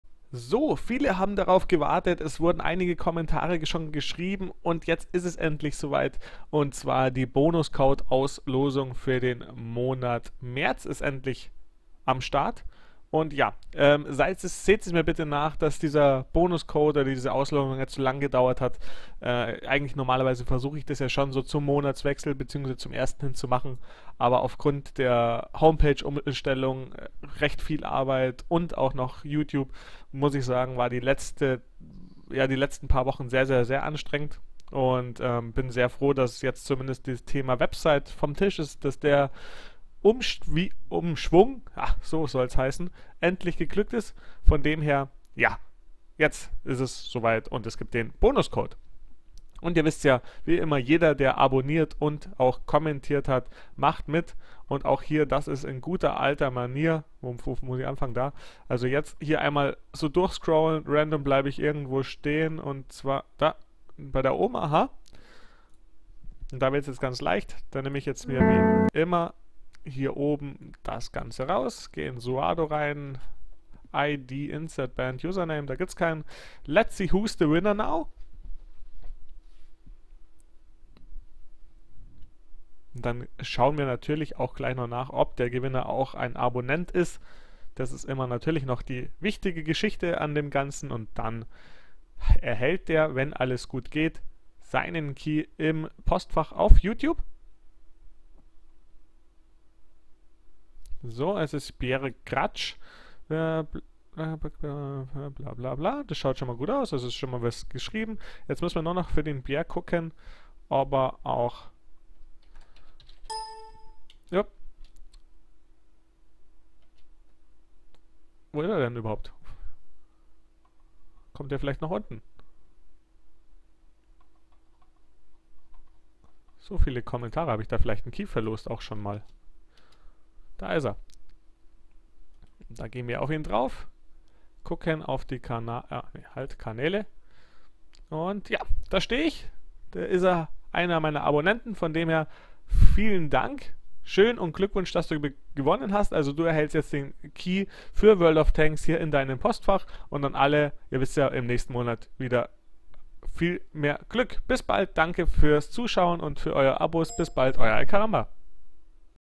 World of Tanks Bonuscode - Abonnenten Gewinner vom März 2015
► Bonuscode für World of Tanks! T-127 + 850 Gold! Jeden Monat als Gewinnspiel.

Nun ist der Monat März rum und wer der Glückliche ist, seht ihr wie immer im Video.

Natürlich habt ihr auch im April wieder die Chance auf einen Code. Einfach jedes Video im Februar mit einem kleinem Text kommentieren und dann einfach Daumen drücken.